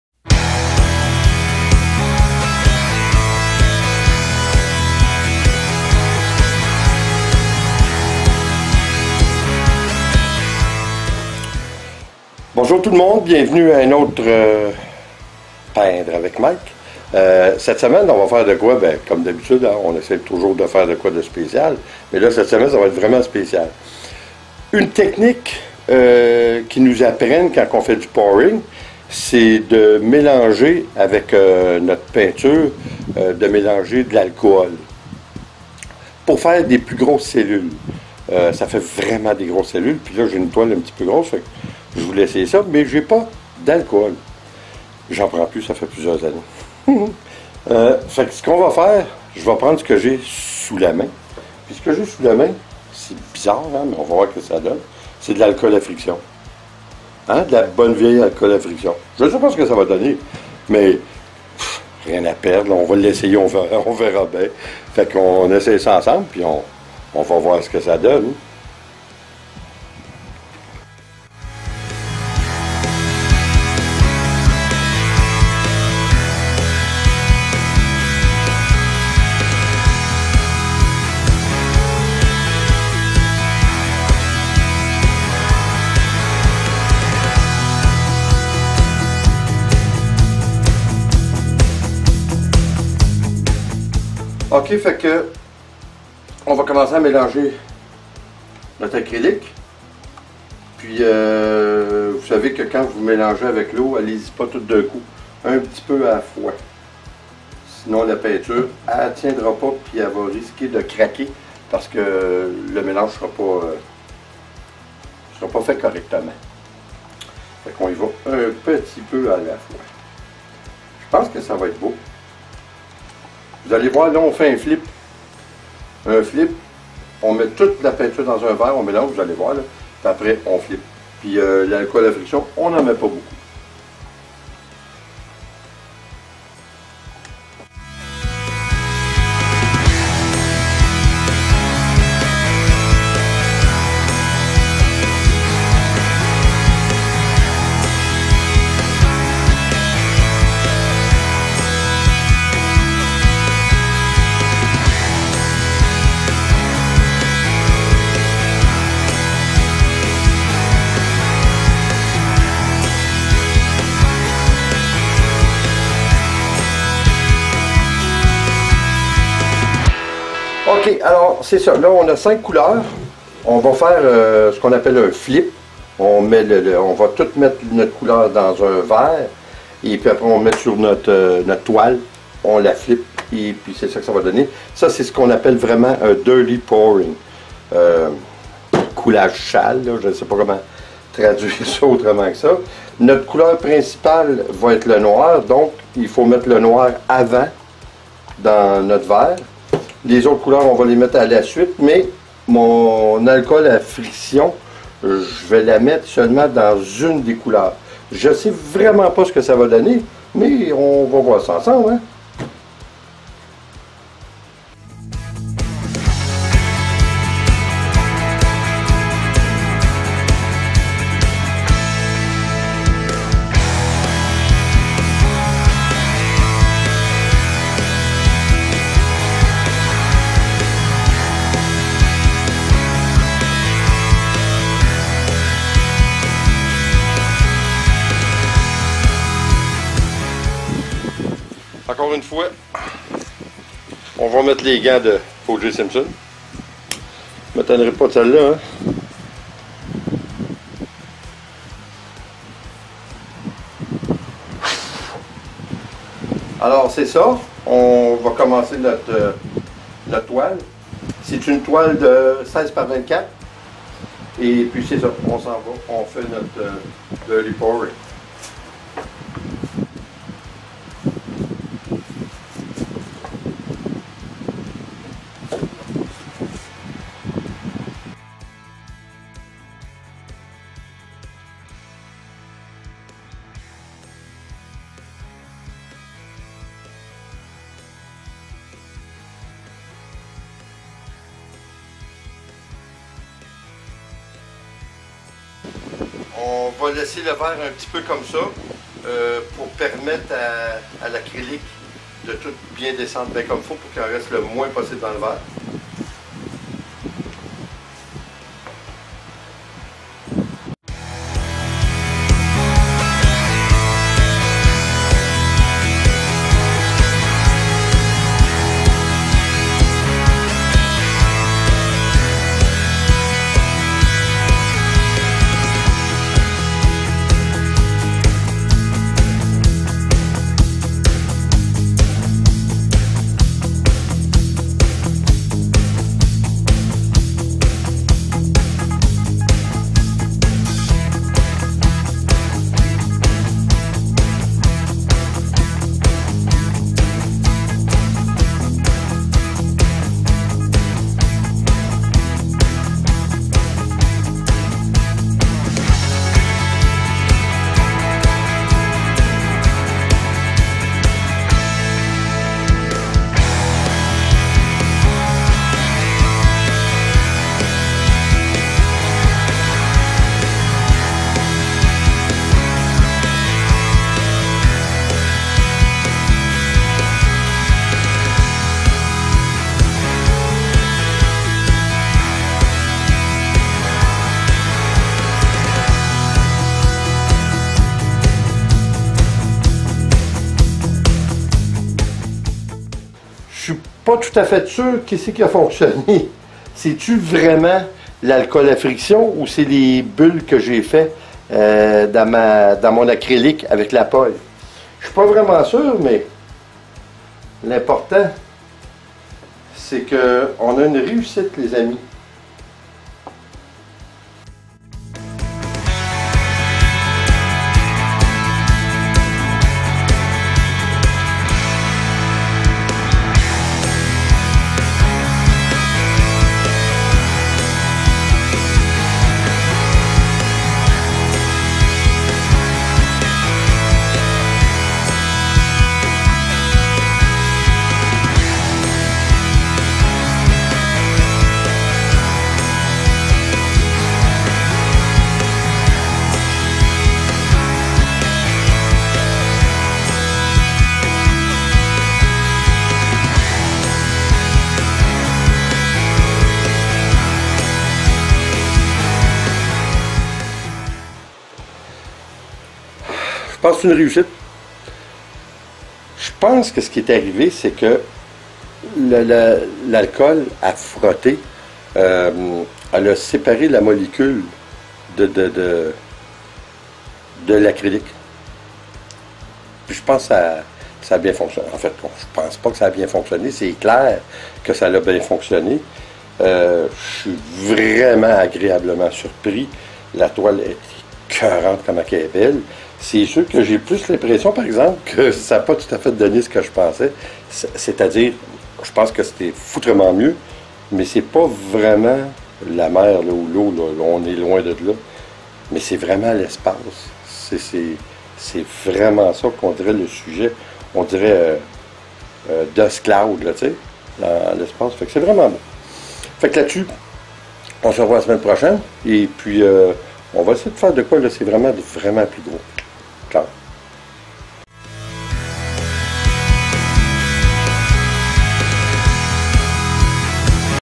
COULAGE A L'ACRYLIQUE *RENVERSÉ* AVEC DE L'ALCOHOL A FRICTION!
Tentative tres réussi de coulage renversé, avec un ajout, tres minimum d'alcohol a friction, normalement on obtiens de plus grande cellules avec de l'alcohol, a défaut de quoi j'ai risquer celle a friction pour des résultat tout aussi satisfaisant!

Seule petit hick, a l'avenir j’aurais mis un peut plus d'acrylique, le résultats est superbe mais la quantité fut tres juste, enfin, pour une toile de 16 x 24 !